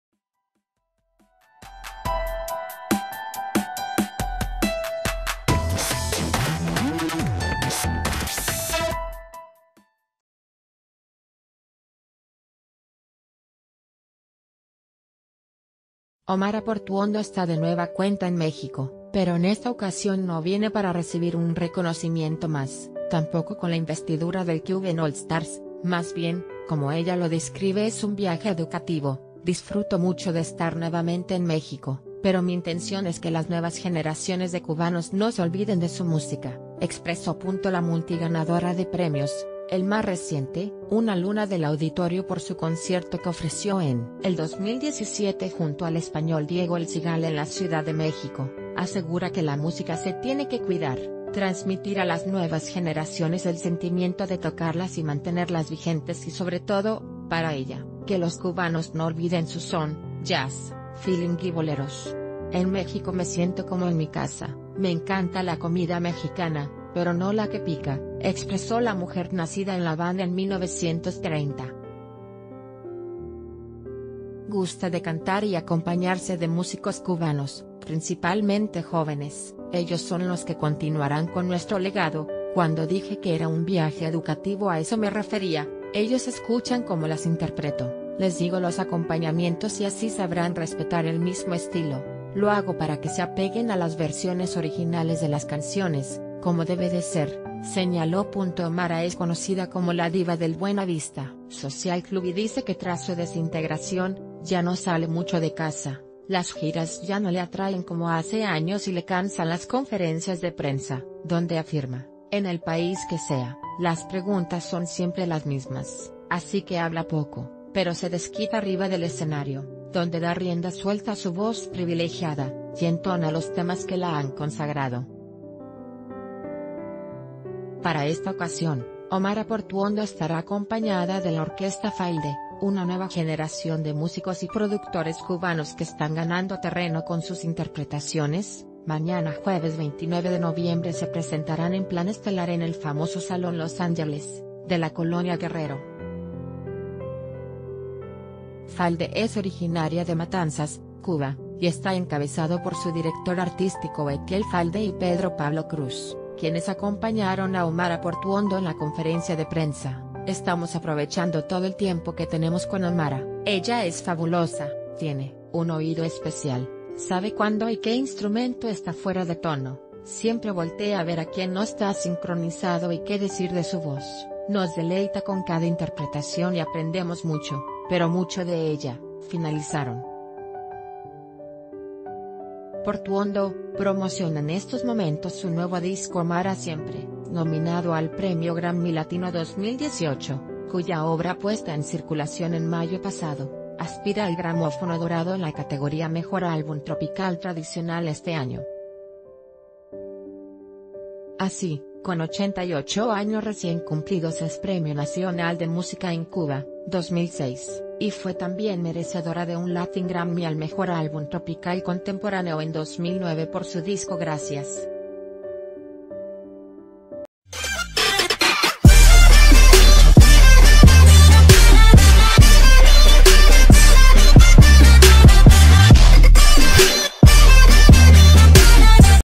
Omara Portuondo regresa a México en el legendario Salón Los Ángeles
Omara Portuondo está de nueva cuenta en México, pero en esta ocasión no viene para recibir un reconocimiento más, tampoco con la investidura del Cuban All Stars, más bien, como ella lo describe “es un viaje educativo, disfruto mucho de estar nuevamente en México, pero mi intención es que las nuevas ...